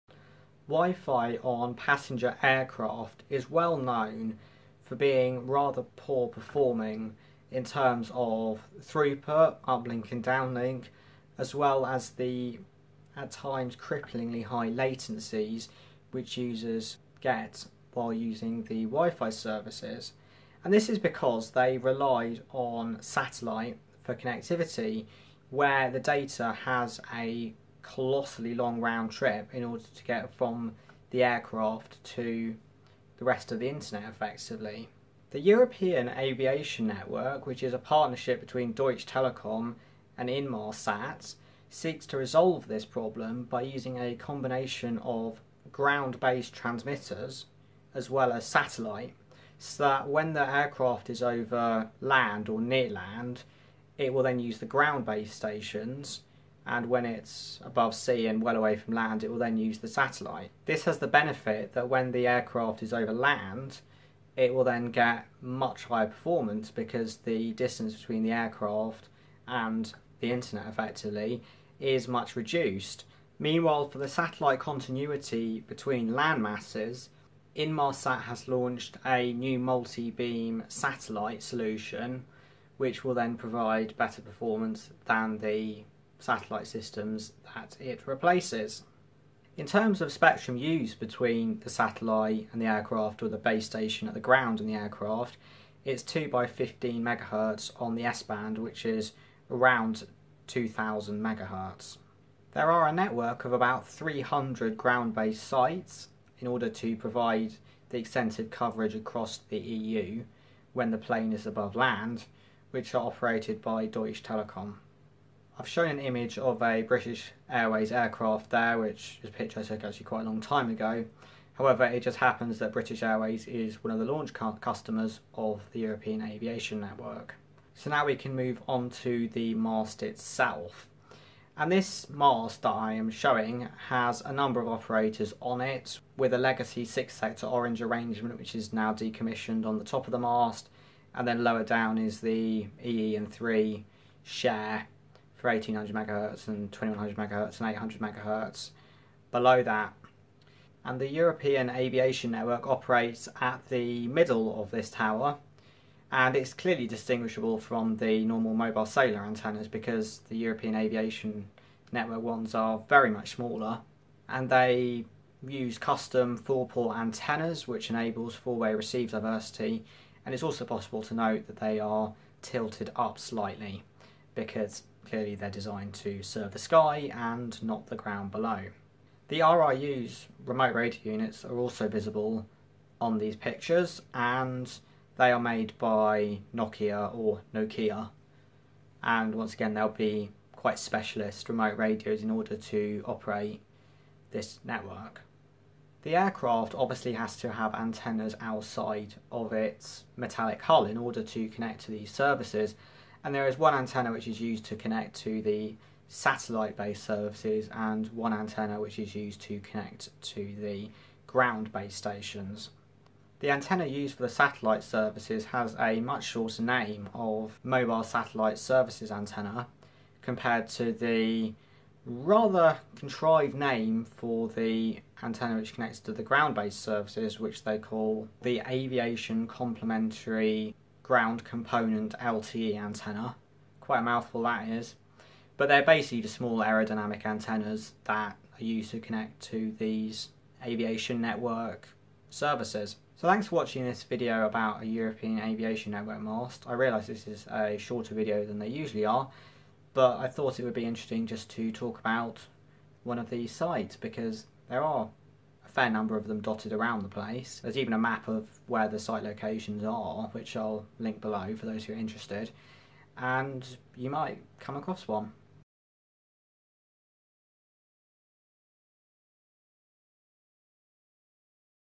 European Aviation Network (EAN) Explained with Ground Based Mast Example
The European Aviation Network, a partnership between Deutsche Telekom and Inmarsat, aims to improve the in flight networking experience through the use of a combination satellite and ground based mast solution. I explain about the network and the ground based masts